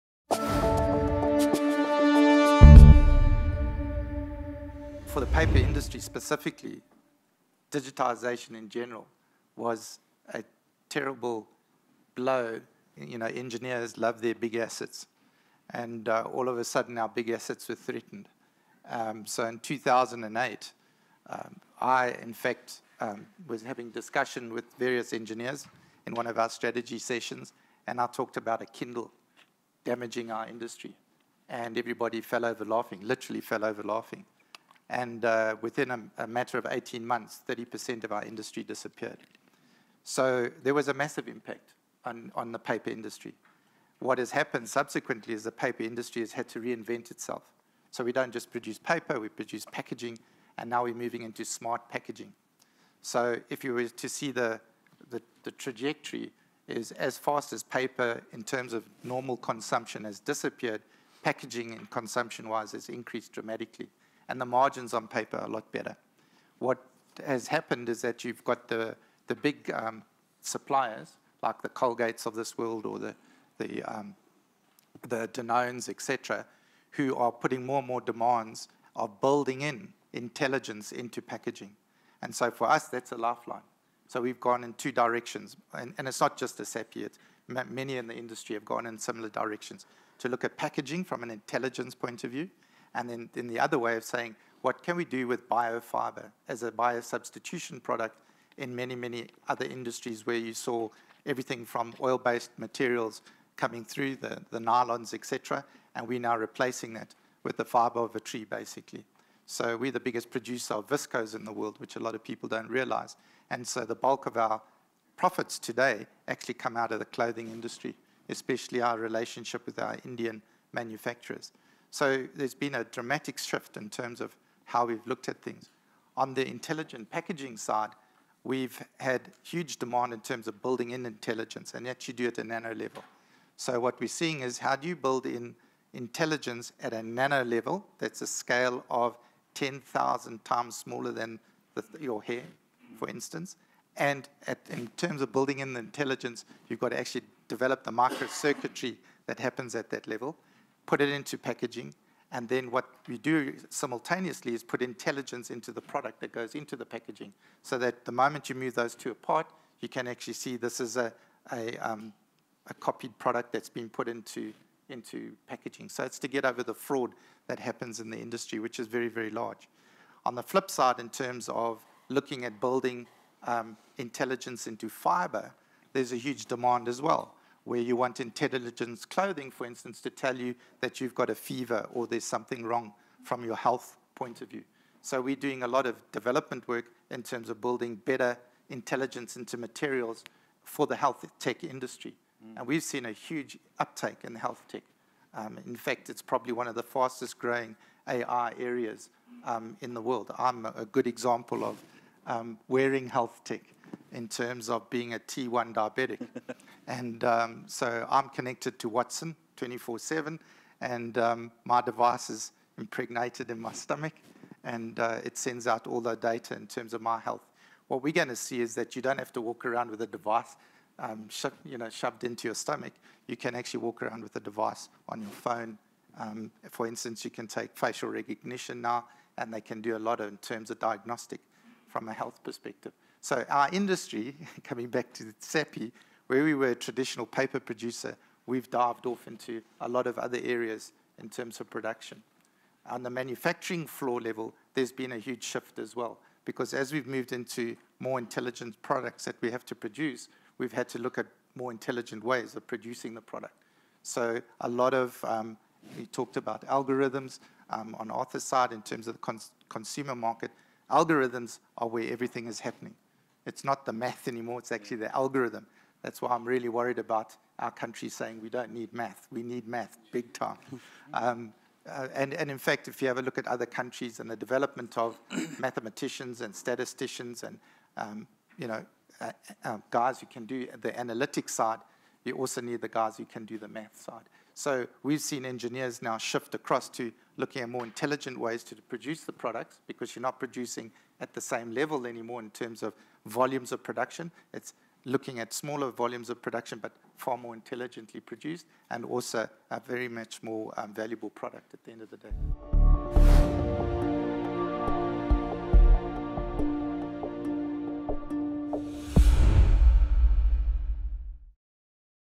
Bradley Coward - Artificial Intelligence Transforming Industry
Bradley Coward from Sappi says his industry has to transform and reinvent itself because of AI.